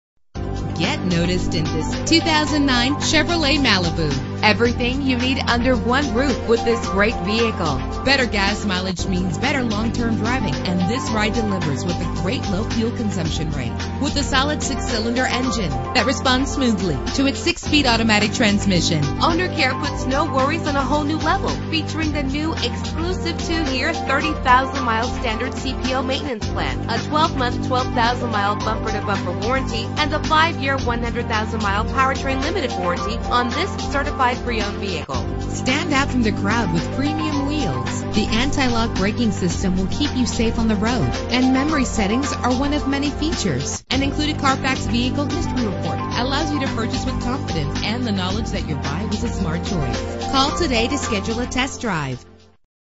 2009 Chevrolet Malibu - Highland MI
Year: 2009
Make: Chevrolet
Model: Malibu
Trim: LTZ
Engine: 3.6 liter 6 cylinder 24 valve MPFI DOHC
Transmission: 6-Speed Automatic
Color: Black Granite Metallic
Mileage: 41875
Address:
4000 W Highland Rd